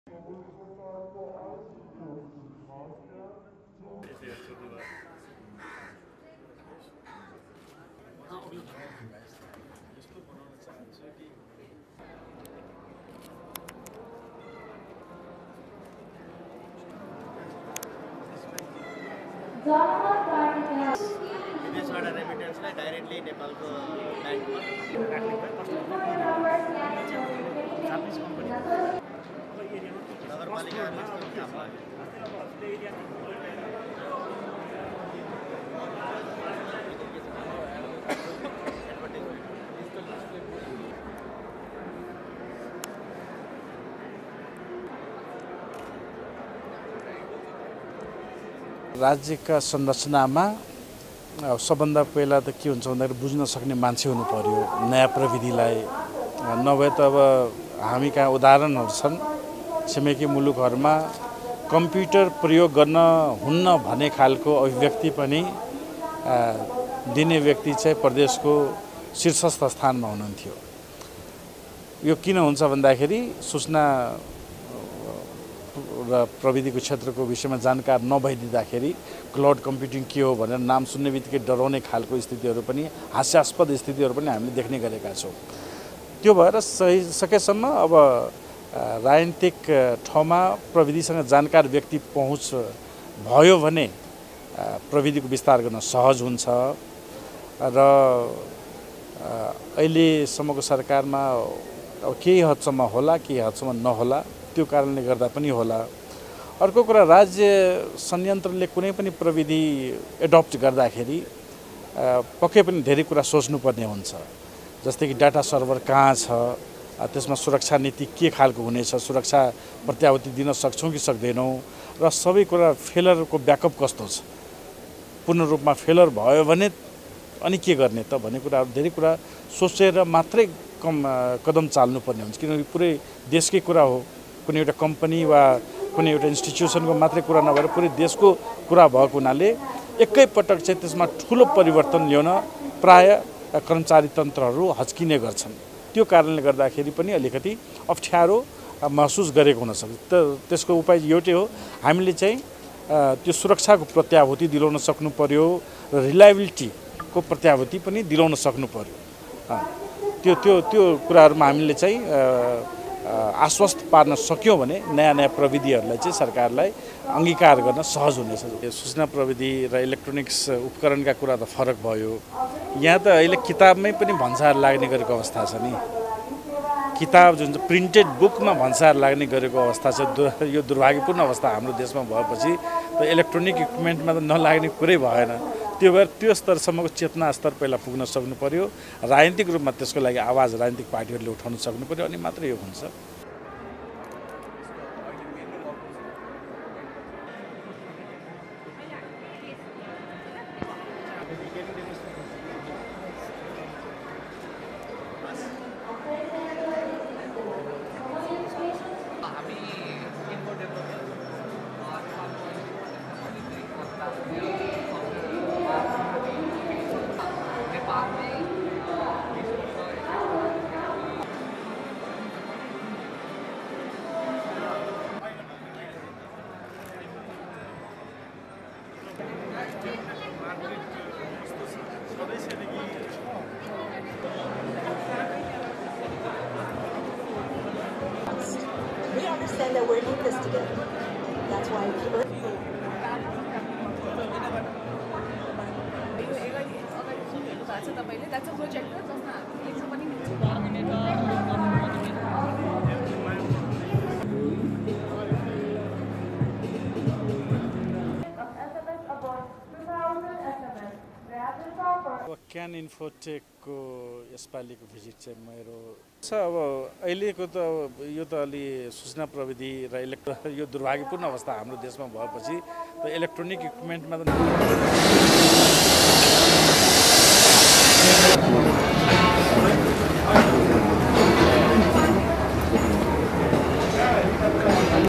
राज्य संरचनामा नयाँ प्रविधिलाई बुझ्ने मान्छे हुनुपर्छ डा सिके राउत
राज्य संरचनामा नयाँ प्रविधिलाई बुझने मान्छे हुनुपर्छ , डा. सिके राउत